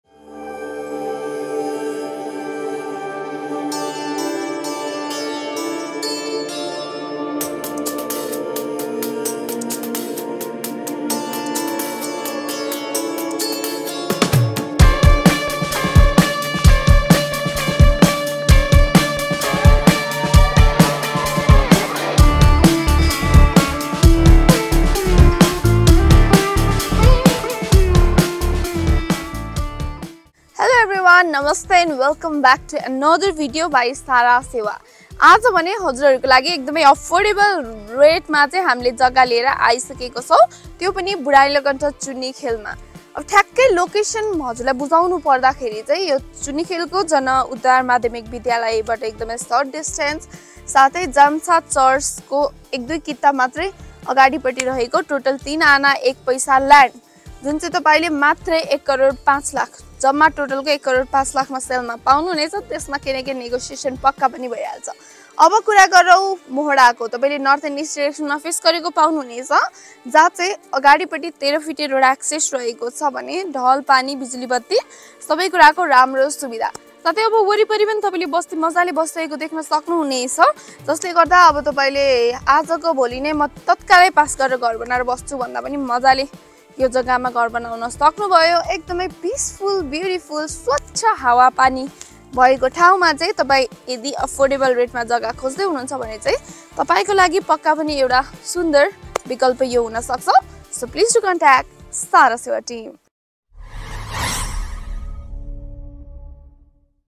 Perfect 3 Aana 1 Poisa Land On Sale At Budhanilkantha Kathmandu.
🎀 Perfect Land On Sale At Budhanilkantha Kathmandu.

Property CODE

Features:

- 3 Aana 1 Paisa Land Area

- North East Facing

- Good Road Access [13 Feet]

- 100 Meter from Main Road

- Center Area

- Good for Residential

- All Amenities Available

- Parking Available

- Finance Available

- All Commercial Needs nearby

- Public Transport Nearby

- Hospital | School | Transportation | Highway nearby

- Water | Electricity | Drainage Available

- Peaceful Environment

- Perfect for Investment

- Affordable Rate
यदि तपाई पनि यसरीनै आफ्नो Property को बिज्ञापन गर्न चाहनु हुन्छ भने हामीलाई सम्पर्क गर्नुहोस.
SaraSewa.com
Ratopul, Kathmandu